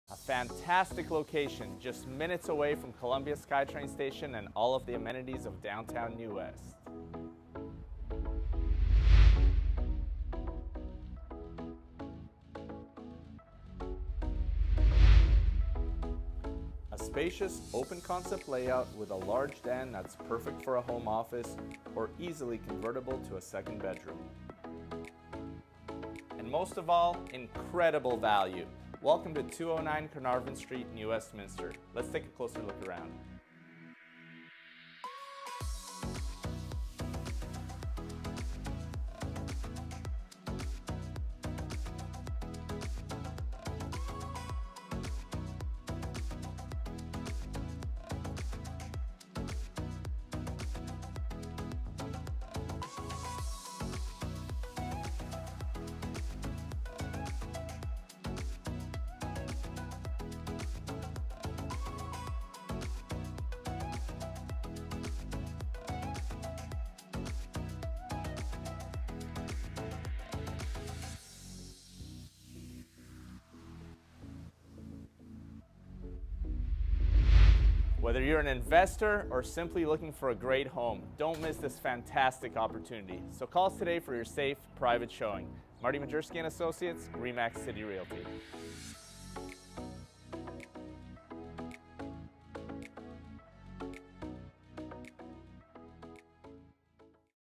205 - 209 Carnarvon St, New Westminster
ATTN: INVESTORS and FIRST TIME BUYERS! Welcome to Argyle House, conveniently located a short 4 MIN WALK to Columbia SKYTRAIN STATION. This home features an INCREDIBLY SPACIOUS OPEN CONCEPT floor plan, new VINYL PLANK flooring throughout, a BRAND NEW full sized L/G washer & dryer, and a HUGE DEN that can easily be converted to a 2ND BEDROOM. Enjoy all the conveniences of downtown New West just minutes walk from your front door including a short 5 minute walk to WESTMINSTER PIER PARK and the Boardwalk. This home comes with 1 PARKING, a STORAGE LOCKER, and strata fees include HEAT & hot water. RENTALS are fully allowed, making this a great INVESTMENT property. Recent building upgrades include a full parkade membrane project that has recently completed. Don't miss this INCREDIBLE VALUE.